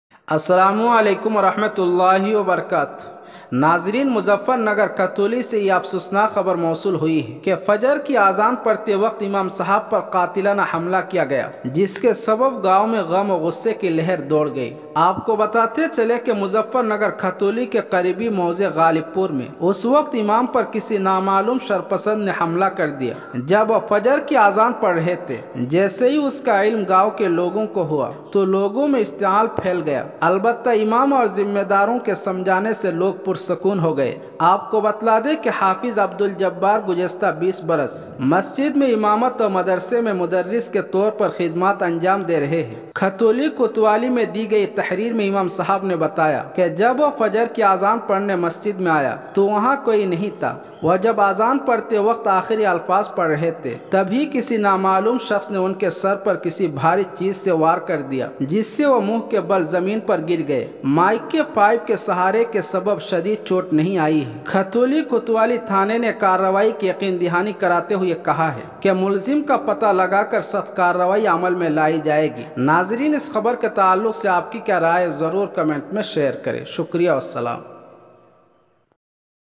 May 5, 2023 مظفر نگر فجر کی اذان پڑھتے وقت امام صاحب پر حملہ 😢
مظفر نگر میں اذان پڑھتے وقت امام صاحب پر جان لیوا حملہ
video
imam Shaib
imam Shaib par hamla